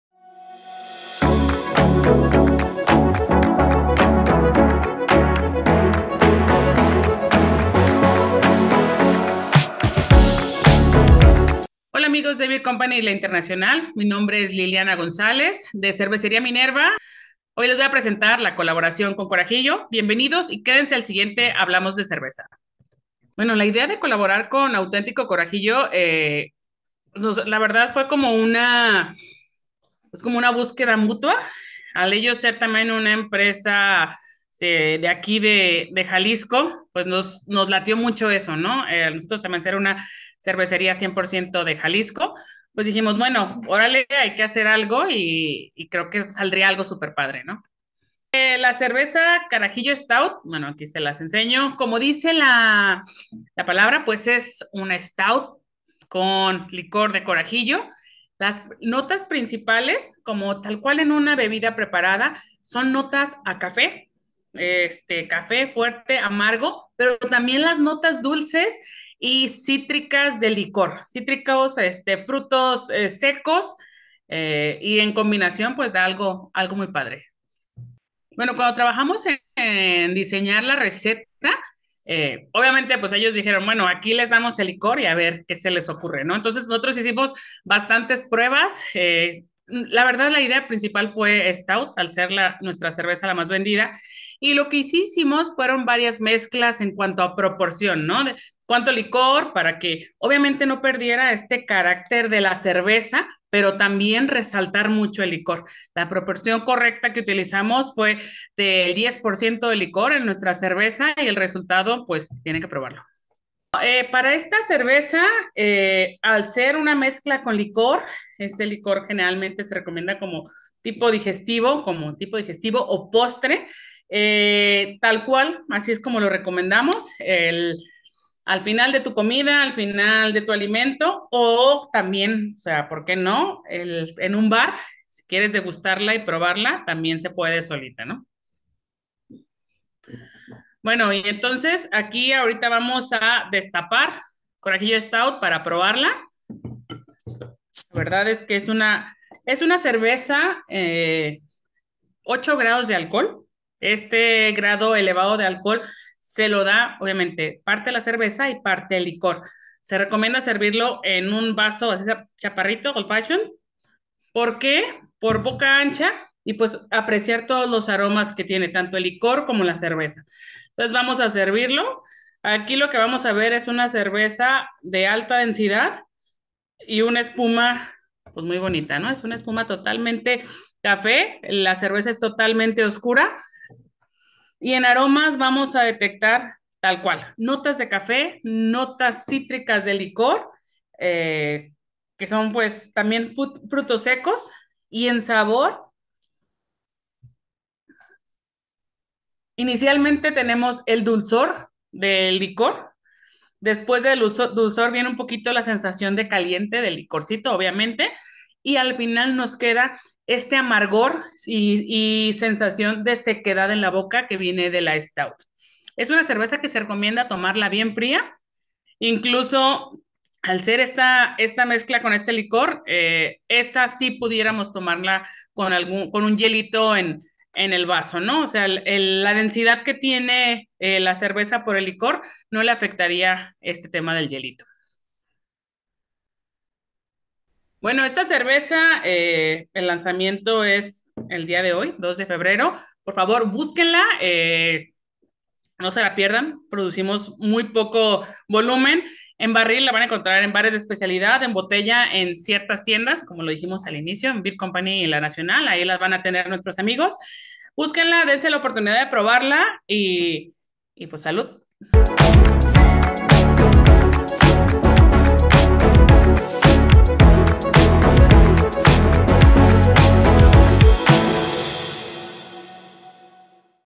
Hablamos Cerveza Carajillo Stout
Carajillo Stout - Cervecería Minerva

Liliana González maestra cervecera de cervecería Minerva nos cuenta de su colaboración con auténtico corajillo que dio como resultado una gran cerveza con aroma y cuerpo con maltas tostadas y un par de toques a naranja y vainilla con  8% de alcohol.

¡Destapa una carajillo stout y dale play!